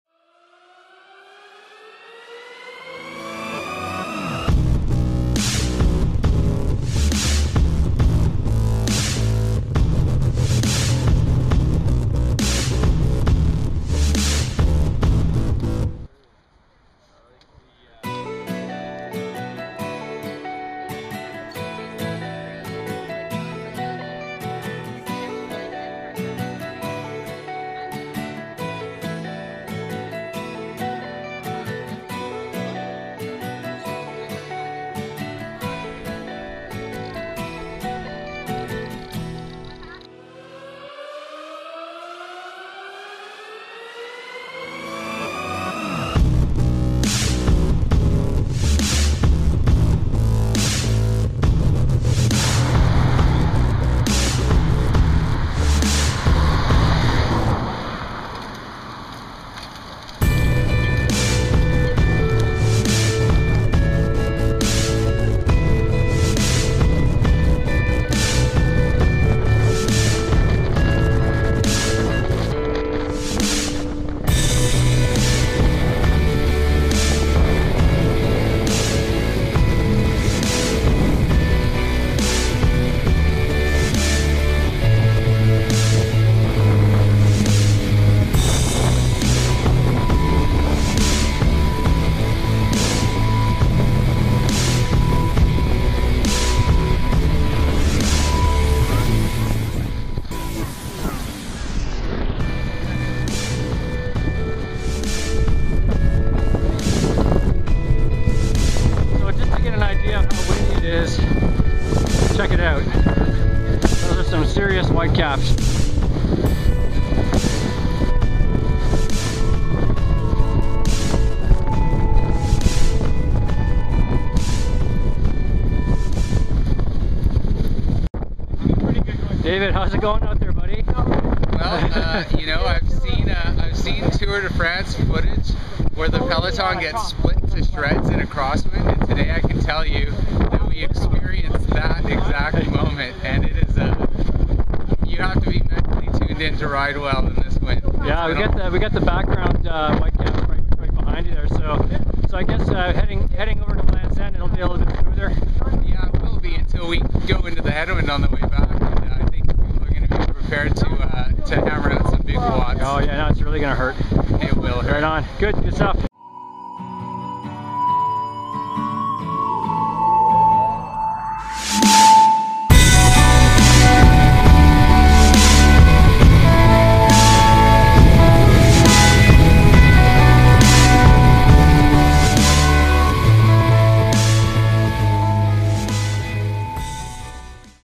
Russ Hay's / Accent Inns Cycling Team - Training Camp Video Blog - Part 2
Recap of Day 3 of the Russ Hay's Accent Inns Cycling Team Winter Training Camp powered by the Pacific Cycling Centre.